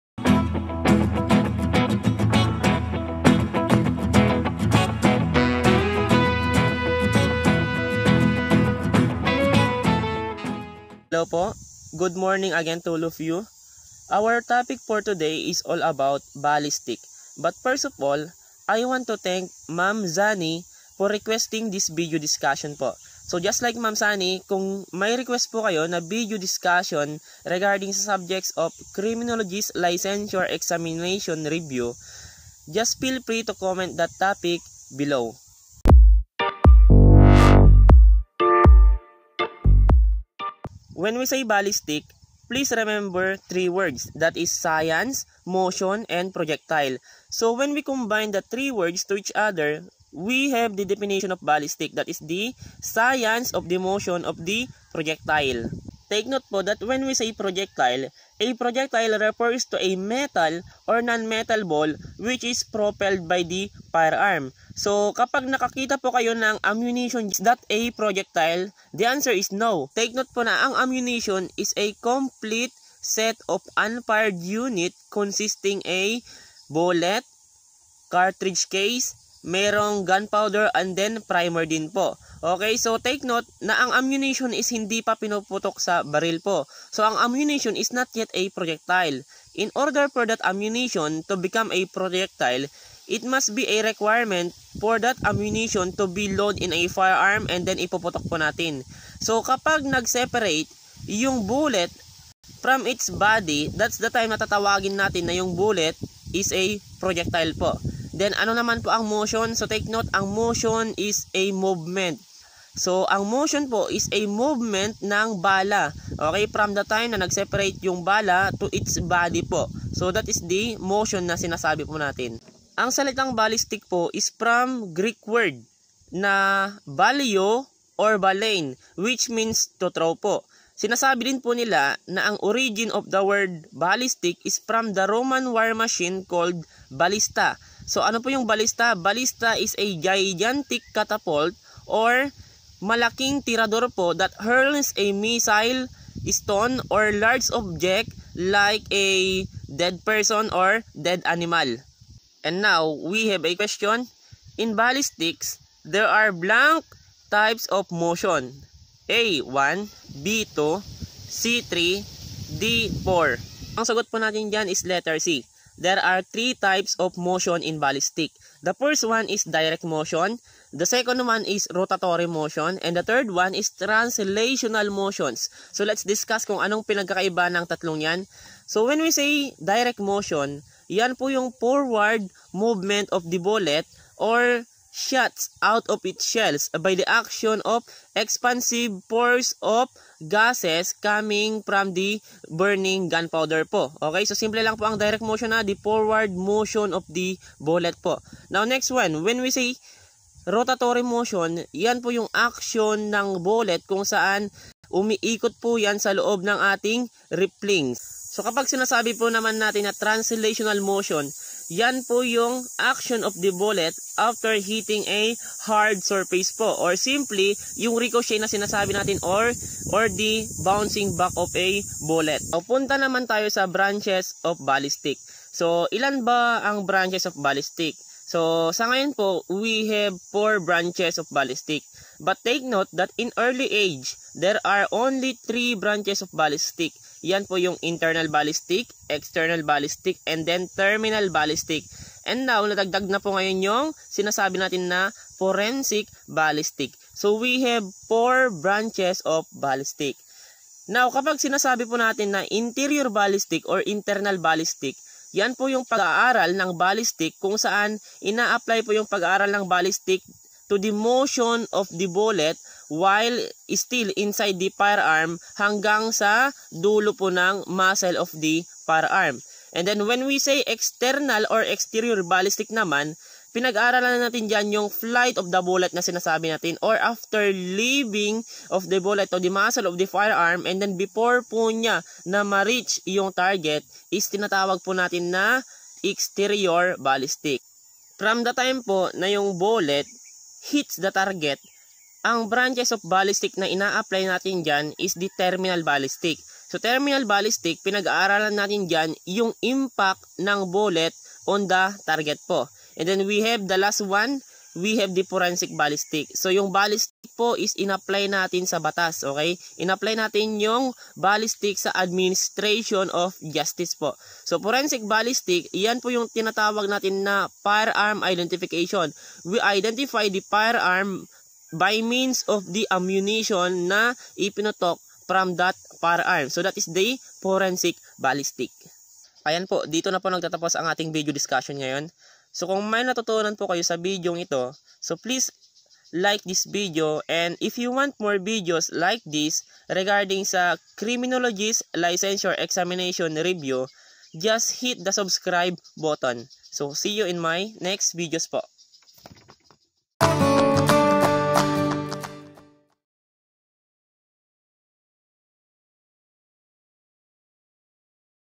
Ballistic lecture 1 (Criminalistics) Criminology Board Exam review
What is Ballistic. Types of Motion. Different branches of Ballistic.